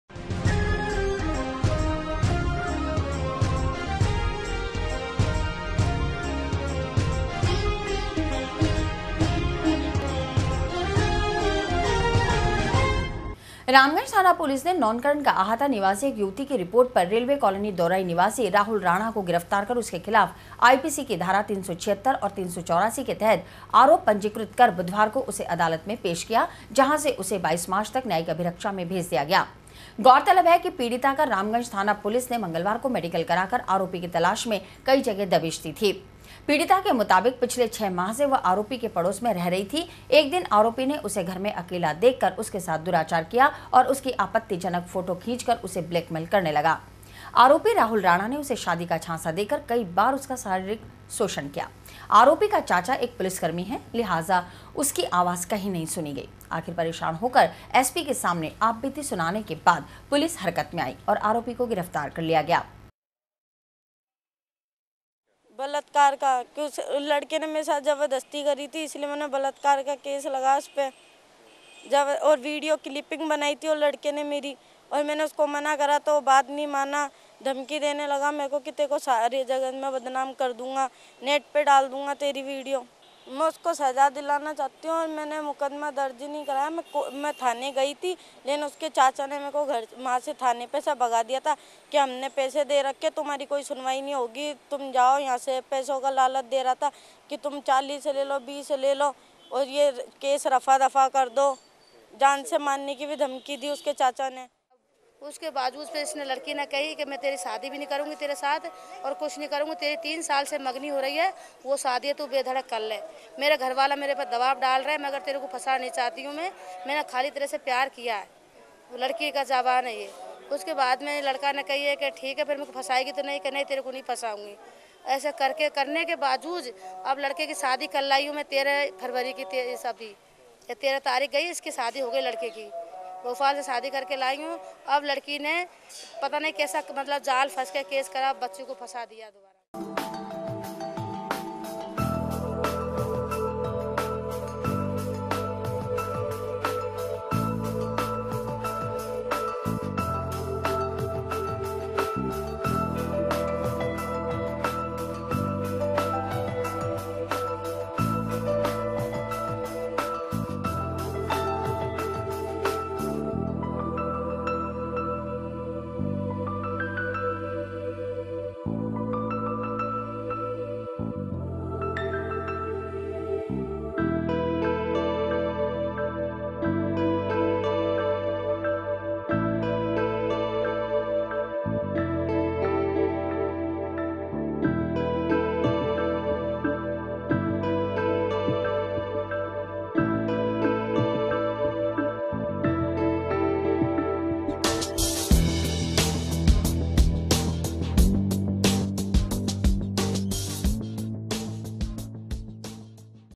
लगाया पड़ोसी पर दुराचार का आरोप
युवती ने लगाया पड़ोसी पर दुराचार का आरोप। पीडि़ता कराया मेडिकल। आरापी को गिरफ्तार कर पेश किया न्यायालय में। 22 मार्च तक भेजा जेल।